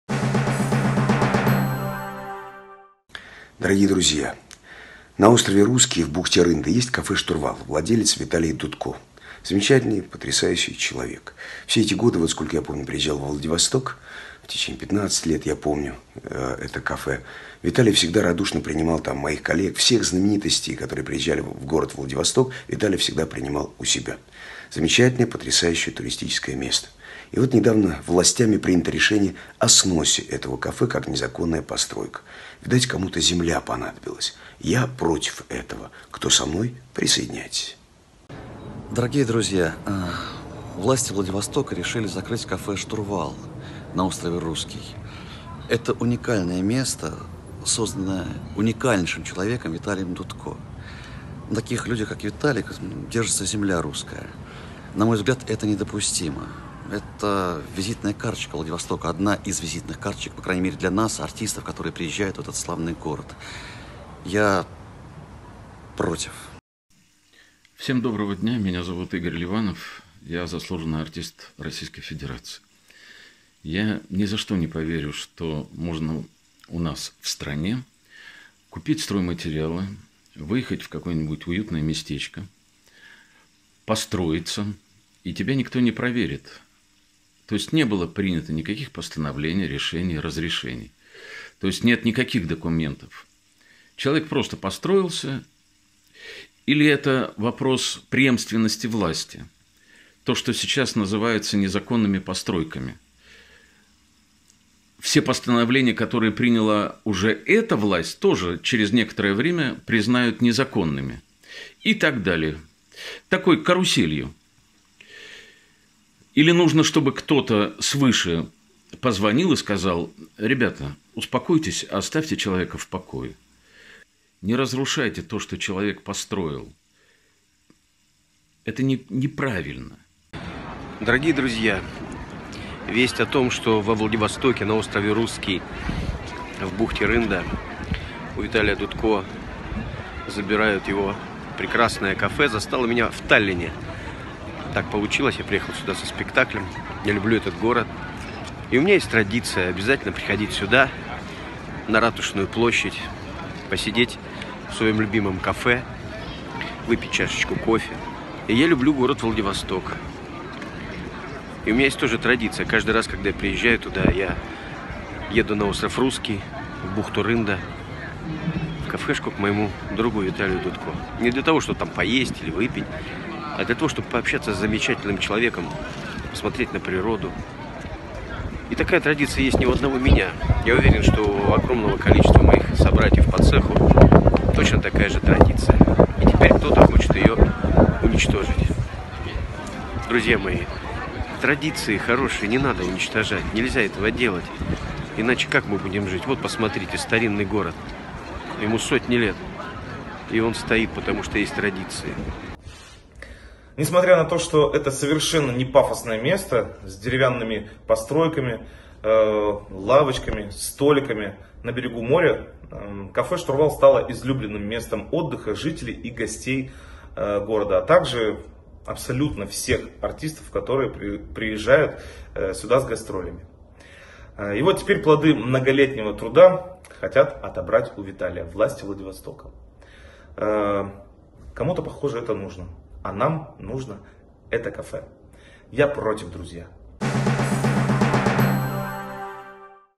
VL.ru - Безруков, Сумишевский и Ливанов выразили протест против сноcа кафе во Владивостоке
Новость о планах мэрии Владивостока снести кафе "Штурвал" на острове Русский, где принимали отечественных звезд, депутатов и угощали Владимира Путина, вызвала обеспокоенность у отечественных деятелей культуры. Артисты Сергей Безруков, Андрей Кайков, Игорь Ливанов, Игорь Письменный и Ярослав Сумишевский выразили свой протест, запустив интернет-флешмоб.